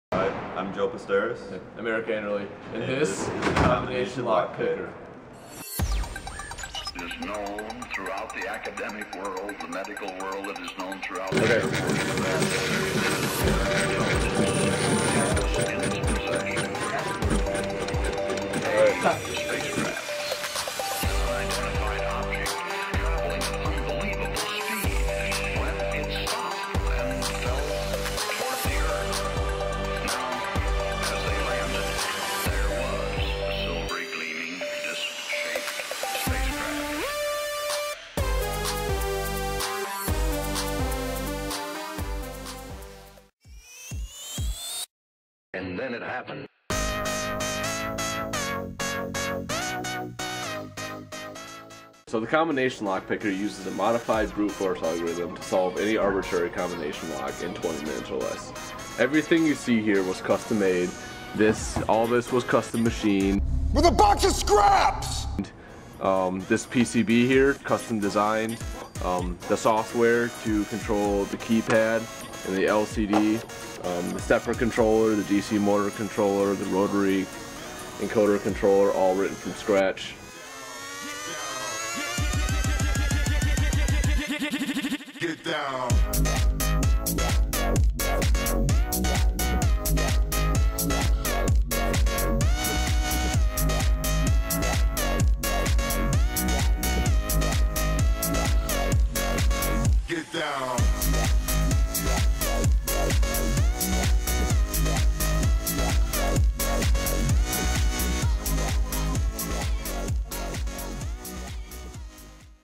Combination Lock Picker - University of Michigan EECS 373 Winter 2012
Our combination lock picker works by using an improved brute force algorithm to test every possible combination of a lock.

The first trick is to realize cheap combination locks have a tolerance of +/- 1 on each of the numbers. This means that even if you are off in either direction by 1, the lock will still open. This cuts the number of combinations we must test from 64,000 to 2,197.

Next, we realized we could avoid re-inputting the first number every time we test a new second number. For instance, we could test 12-10-8 and 12-7-8 without having to re-input 12. This is because the first plate of the lock is not disturbed when you turn the dial back to test the third number if you do not turn the dial past the second number.

Finally, you can test every possible third number without re-inputting the first two. You simply try every third number, and if the combination is right, the lock will open. For example, we can try every combination from 12-10-7, 12-10-6, 12-10-5... all the way back to 12-10-8 without re-inputting the first two numbers.

All of these tricks combined give us a worst-case scenario of any lock that fits into the machine of roughly 20 minutes.